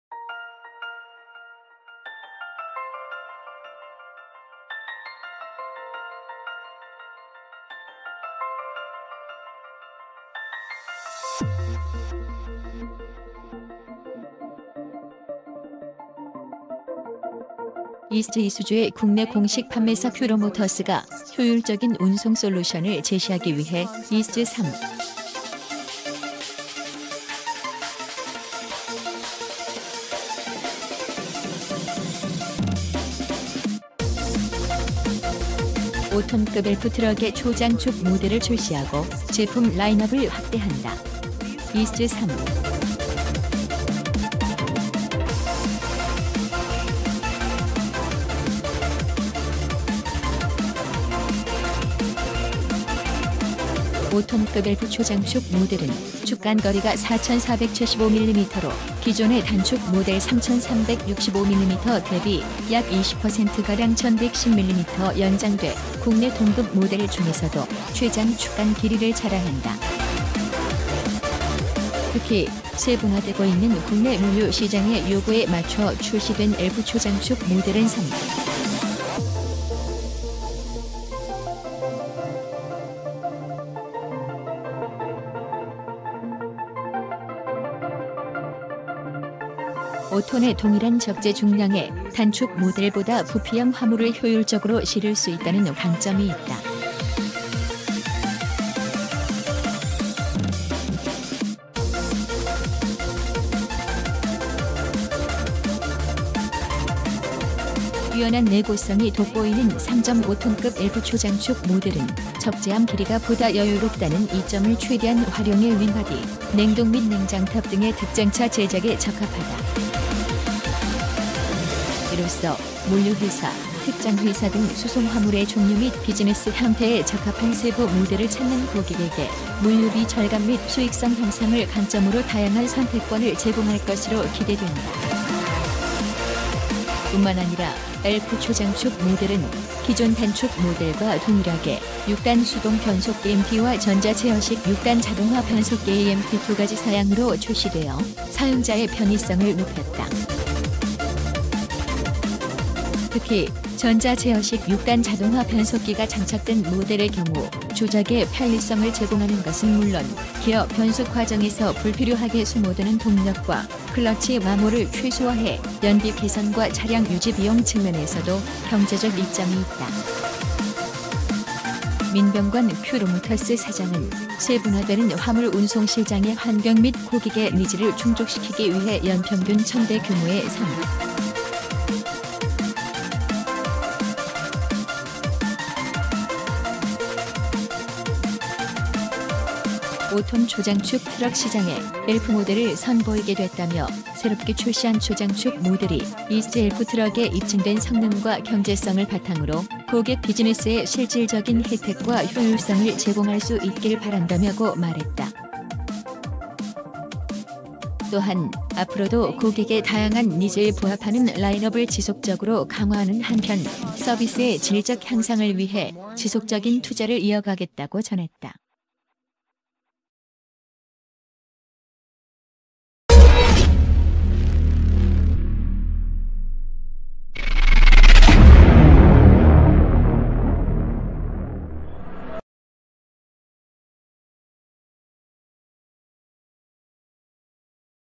큐로모터스, 이스즈 3.5톤급 엘프 트럭 초장축 모델 출시
큐로모터스, 이스즈 3.5톤급 엘프 트럭 초장축 모델 출시 이스즈ISUZU의 국내 공식 판매사 큐로모터스가 효율적인 운송 솔루션을 제시하기 위해 이스즈 3.5톤급 엘프 트럭의 초장축 모델을 출시하고 제품 라인업을 확대한다. 이스즈 3